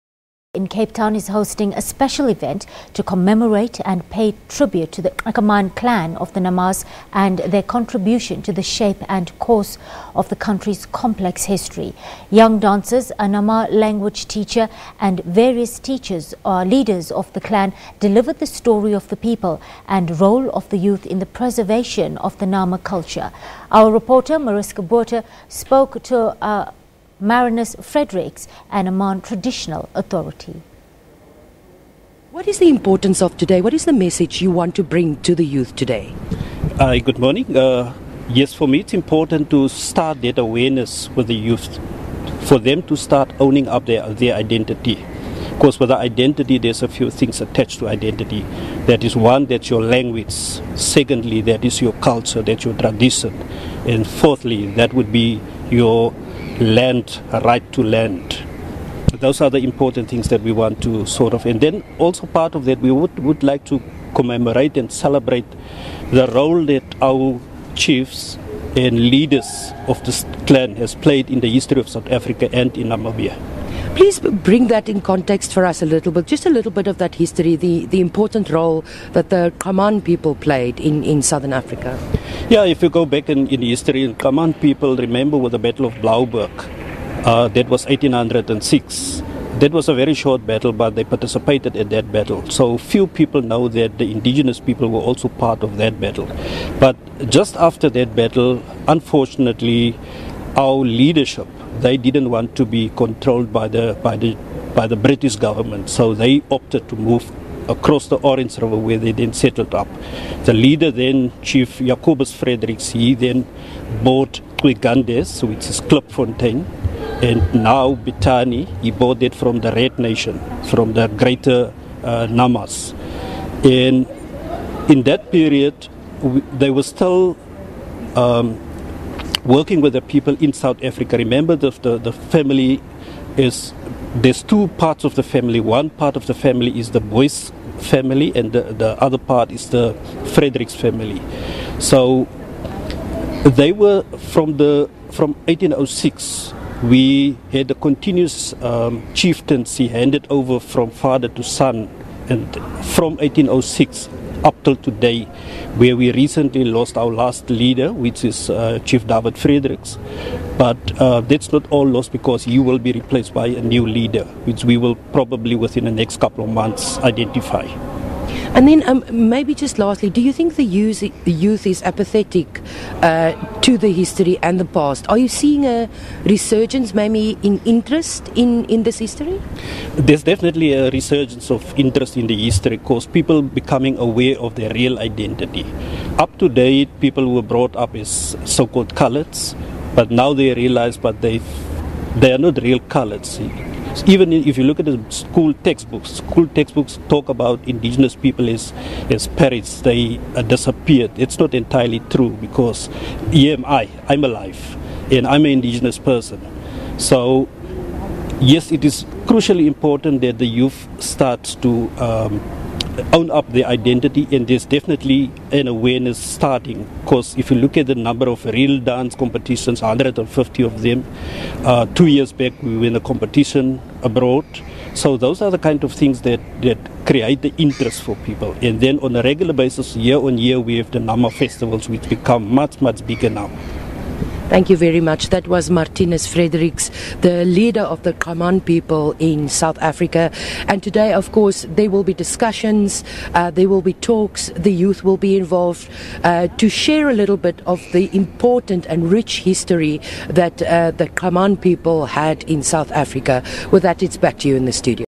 Tribute to the !Aman Nama clan
The Castle of Good Hope in Cape Town is hosting a special event to commemorate and pay tribute the the !Aman Clan of the Namas and their contribution to shape the course of the country's complex history.
Young dancers, a Nama language teacher and various leaders of the !Aman clan delivered the story of the people and role of the youth in the preservation of the Nama culture.
Our reporter Mariska Botha spoke to Marinus Fredericks, Aman traditional authority.